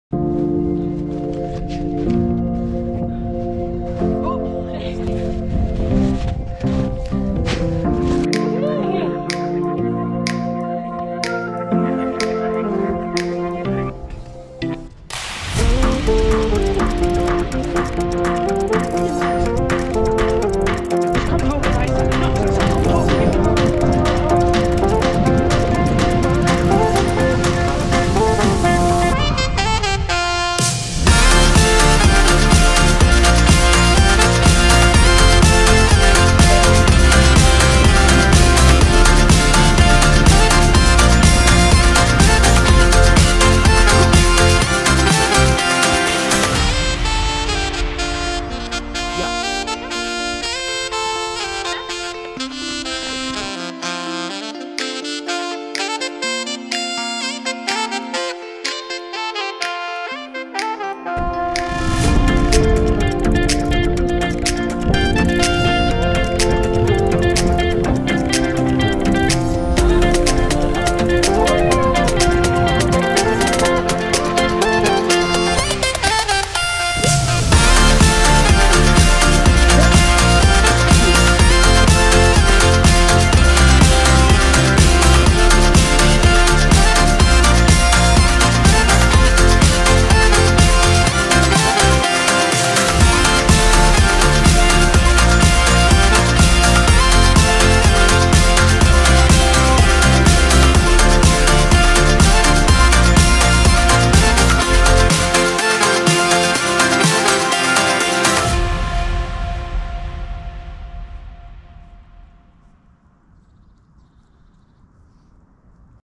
Filmmaking progress GoPro Hero 9
Mein 3. Video mit meiner GoPro Hero 9. Die Edits dauern noch etwas länger aber mein Material wird brauchbarer. Unsere Hündin Crispy bleibt nur ungern im Frame. An Ihren Schauspielerkünsten müssen wir noch arbeiten. Hat aber viel spaß gemacht zu filmen und zu editieren.